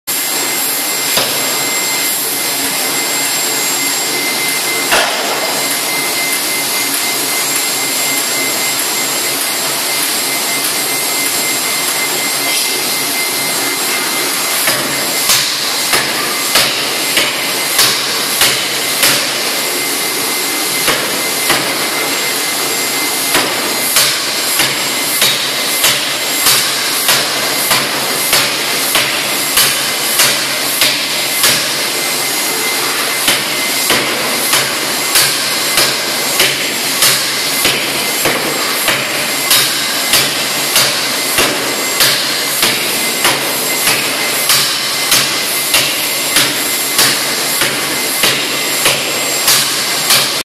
how to high efficiency drawing out on C41 series pneumatic forging hammer
how to high efficiency drawing out on C41 series pneumatic forging hammer,this video will answer you,by the way,the pneumatic forging hammer also used for upsetting,punching,bending,twisting and so on,if you want to know more,please contact me feel free,